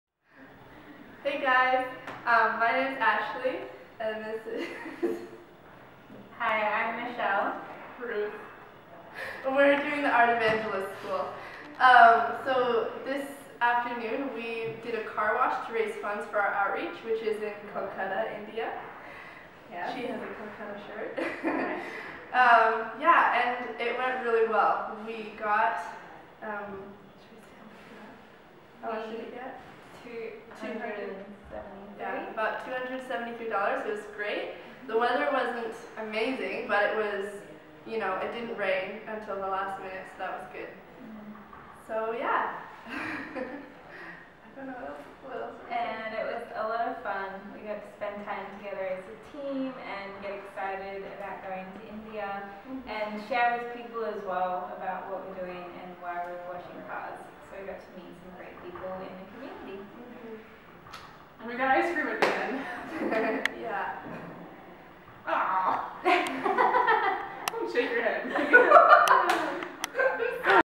YWAM Perth Art Evangelists School Testimony
Some students from Art Evangelists School shared about their fundraising for outreach.